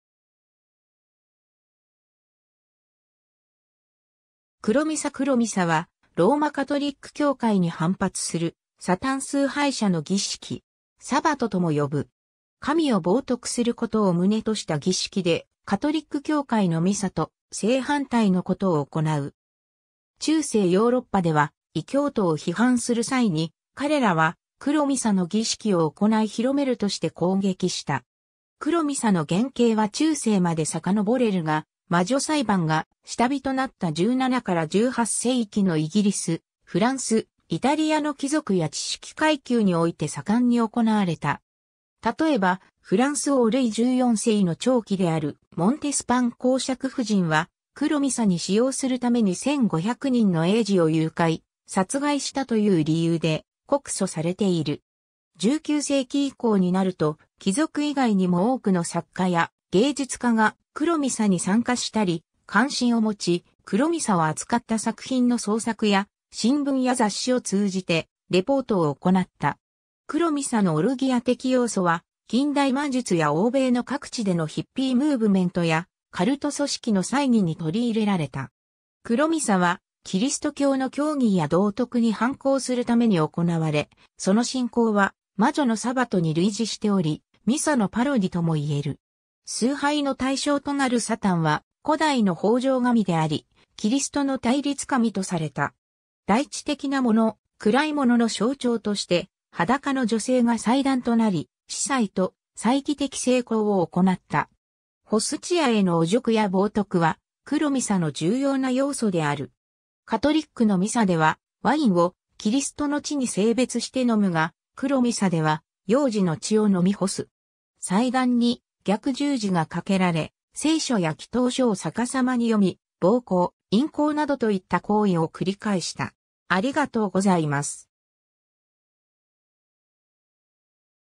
黒ミサ
黒ミサ 黒ミサ（くろミサ）は、ローマ・カトリック教会に反発するサタン崇拝者の儀式。
サバトとも呼ぶ。
神を冒涜することを旨とした儀式で、カトリック教会のミサと正反対のことを行う。
中世ヨーロッパでは異教徒を批判する際に彼らは黒ミサの儀式を行い広めるとして攻撃した。
 黒ミサの原型は中世まで遡れるが、魔女裁判が下火となった17 - 18世紀のイギリス、フランス、イタリアの貴族や知識階級において盛んに行われた。
例えばフランス王ルイ14世の寵姫であるモンテスパン侯爵夫人は黒ミサに使用するために1500人の嬰児を誘拐、殺害したという理由で告訴されている。
 19世紀以降になると貴族以外にも多くの作家や芸術家が黒ミサに参加したり、関心を持ち、黒ミサを扱った作品の創作や新聞や雑誌を通じてレポートを行った。
黒ミサのオルギア的要素は、近代魔術や欧米の各地でのヒッピームーブメントやカルト組織の祭儀に取り入れられた。
 黒ミサはキリスト教の教義や道徳に反抗するために行われ、その進行は魔女のサバトに類似しており、ミサのパロディとも言える。
崇拝の対象となるサタンは古代の豊穣神であり、キリストの対立神とされた。
大地的なもの、暗いものの象徴として裸の女性が祭壇となり、司祭と祭儀的性交を行った。
 ホスチアへの汚辱や冒涜は黒ミサの重要な要素である。
カトリックのミサでは、ワインをキリストの血に聖別して飲むが、黒ミサでは幼児の血を飲み干す。
祭壇に逆十字が架けられ、聖書や祈祷書を逆さまに読み、暴行、淫行などといった行為を繰り返した。